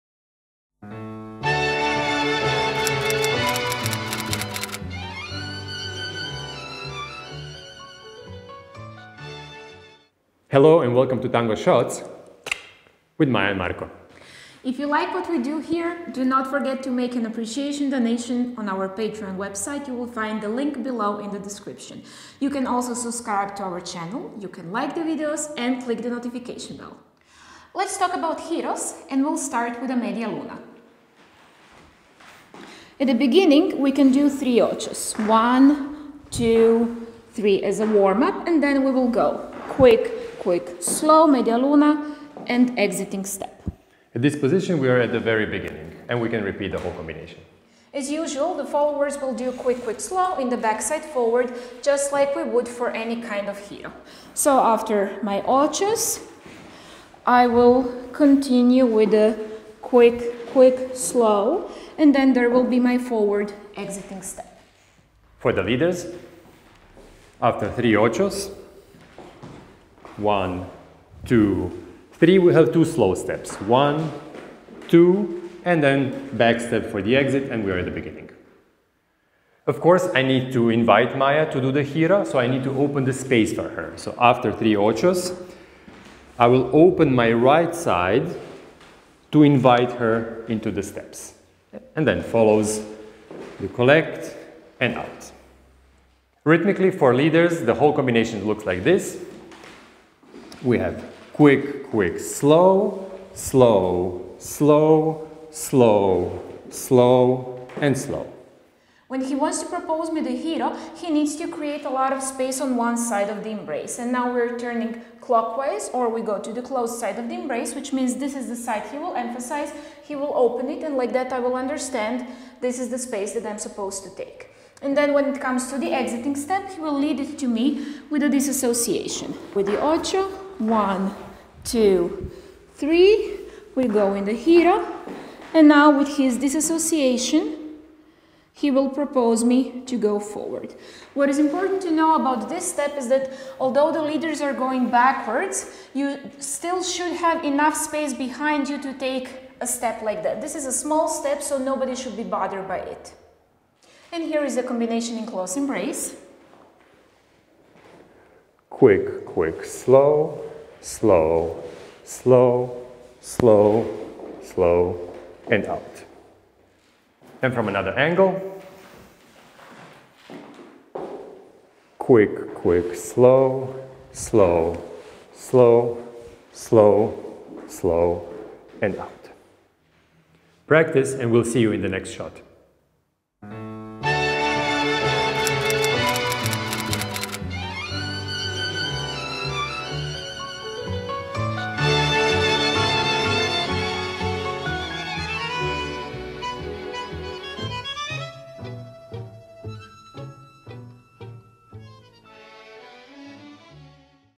Tango Shots with Maja & Marko - #21 Media luna
Dear dancer,

media luna is a great introduction to turns (giros) in tango, and so it's worthwhile to study!

Abrazos,
Maja & Marko